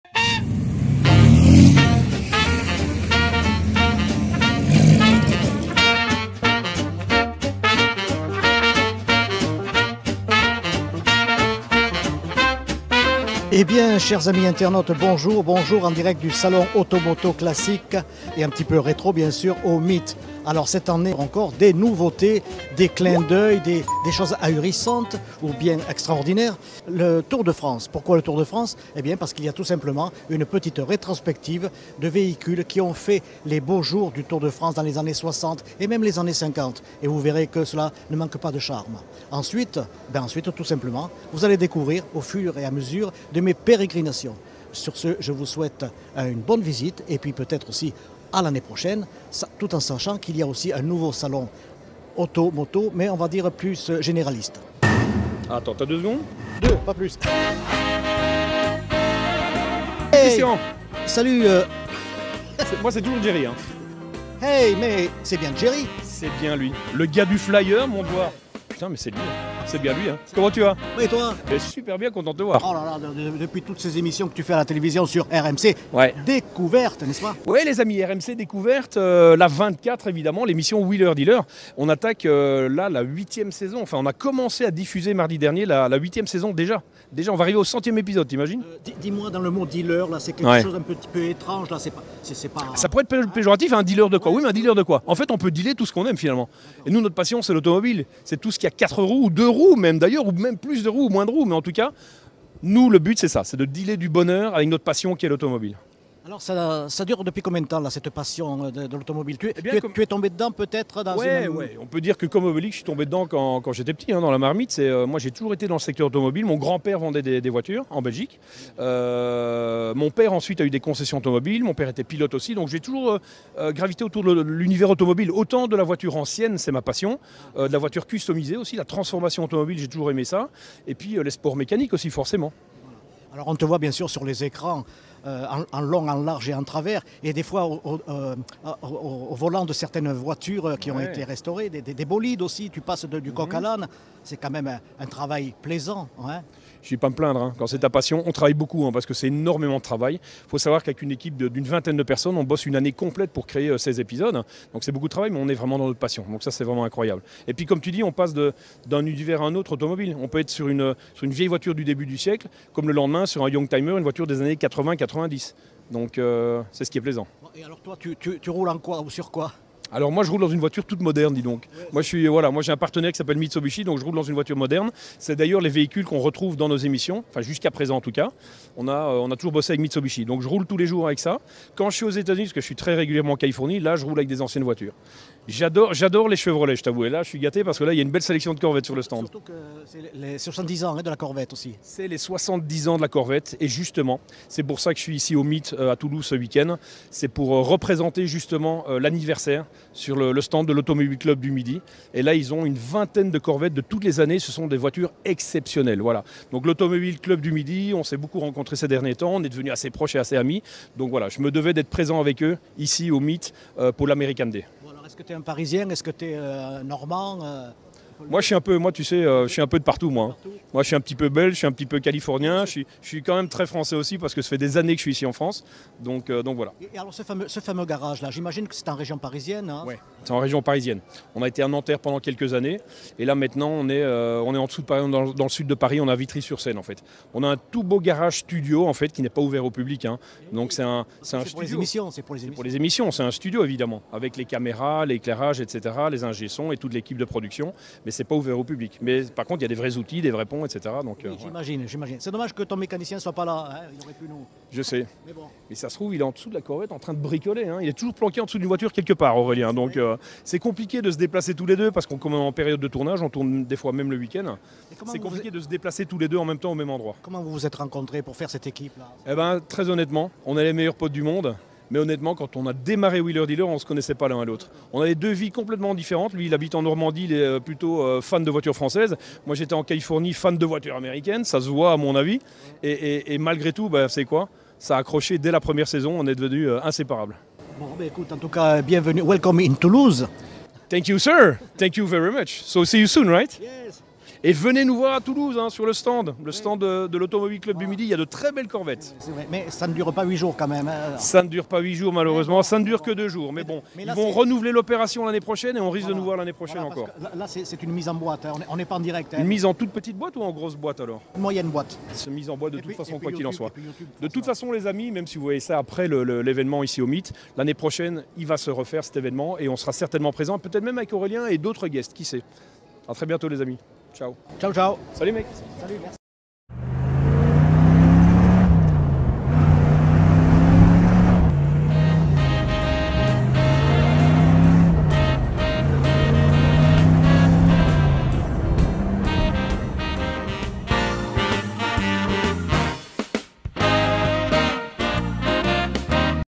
Salon Auto Moto classic Toulouse le Meet 2023
Le rendez-vous annuel du Grand Sud des acteurs du secteur autos-motos anciennes et des passionnés de belles mécaniques s’installait au MEETT, Parc des Expositions .
150 exposants, experts en restauration, entreprises spécialisées, clubs et associations viennent à la rencontre des collectionneurs et de celles et ceux souhaitant acheter, restaurer, équiper un véhicule, trouver de bons conseils ou tout simplement s’évader.
Cette édition marquait aussi le retour de l'American Day, pour deux jours d’ambiance toujours plus festive avec concerts sur scènes et shows de danse. plusieurs  videos  à suivre  sur  le sujet ...